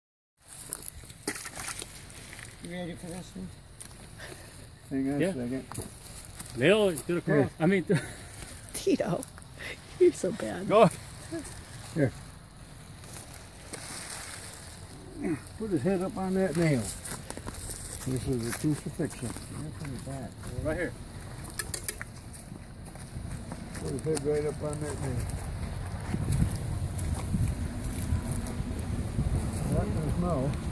Mom wanted to help with the cleaning of the fish! D'LO, Mississippi
Back in the day, my mom was one of the few women that knew how to clean the fish. Priceless watching her being a part of it here.